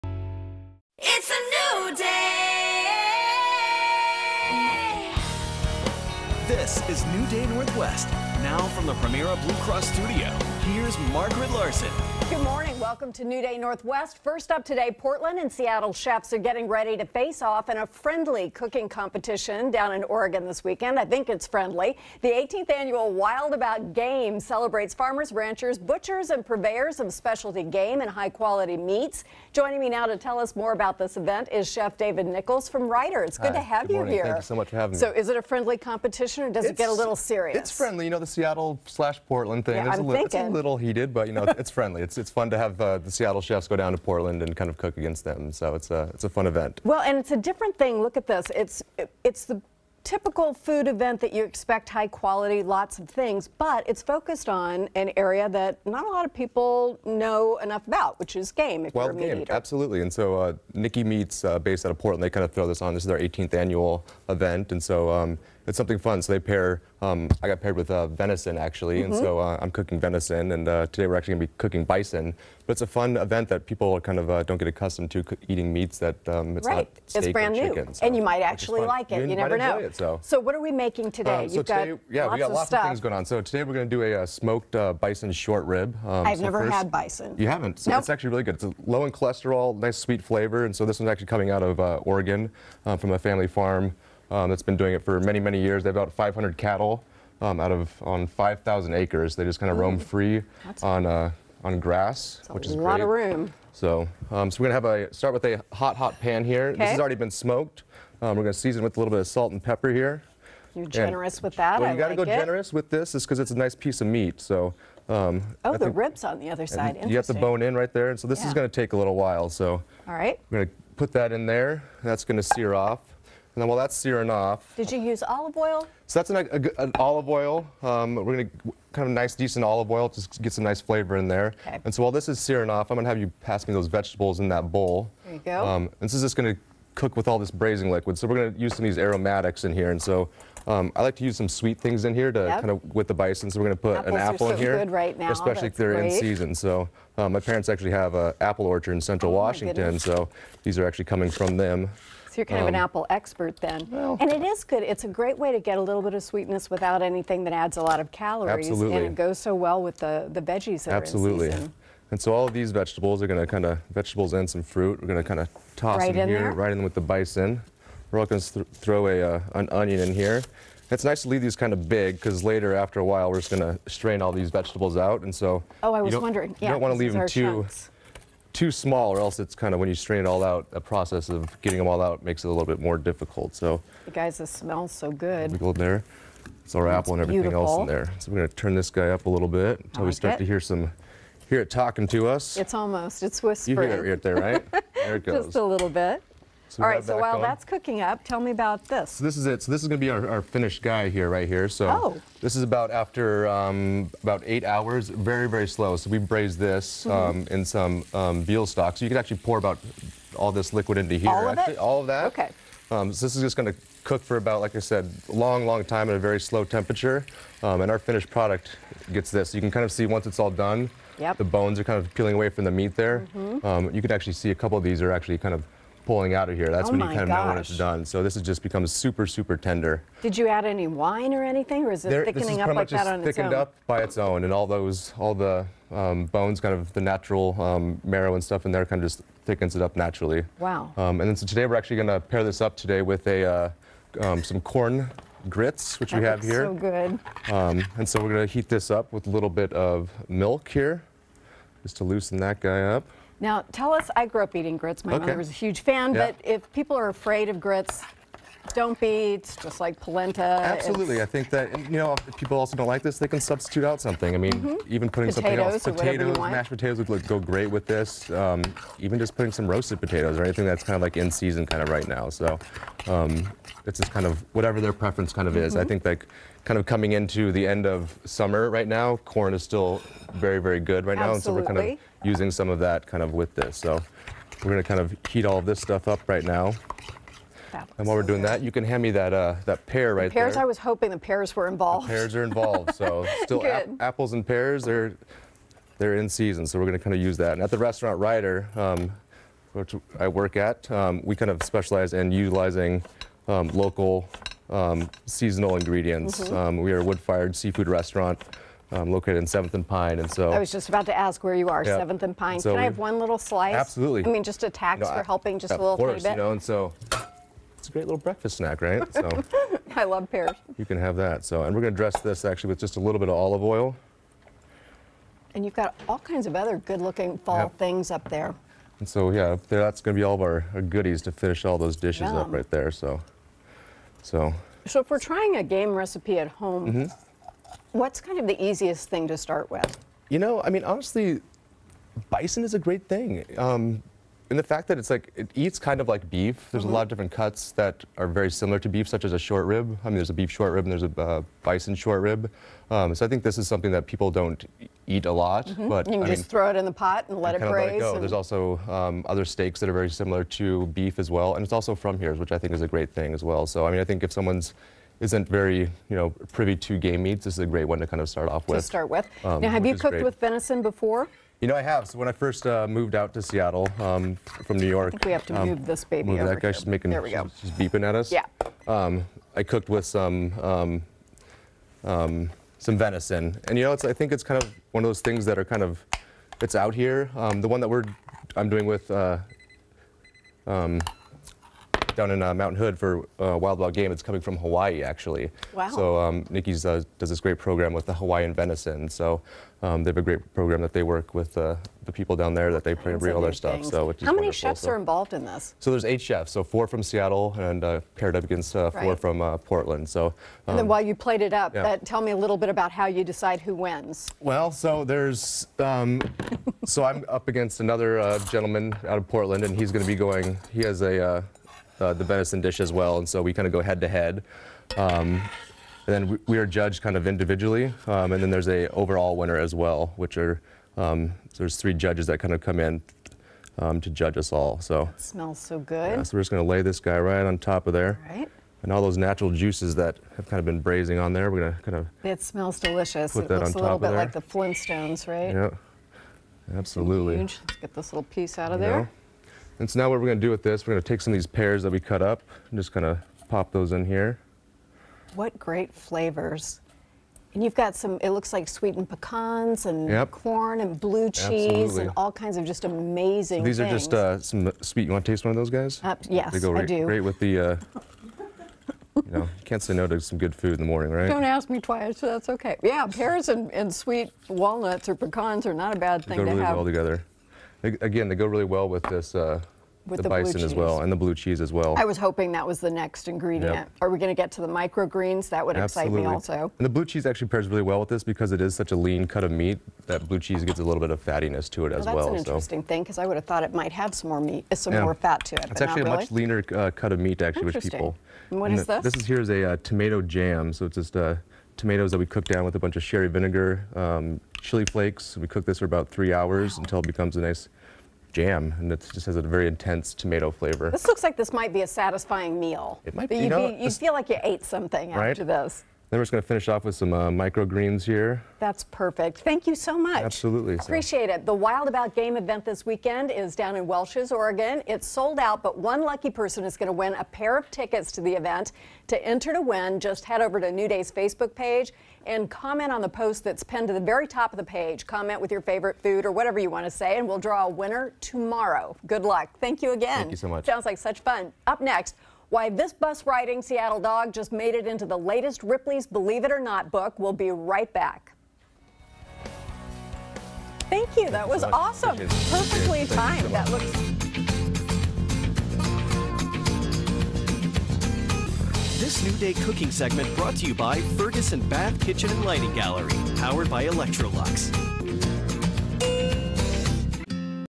Wild About Game chef David Nichols makes a meaty dish
David Nichols, a chef who is going to compete in Wild About Game, cooks a tasty bison dish.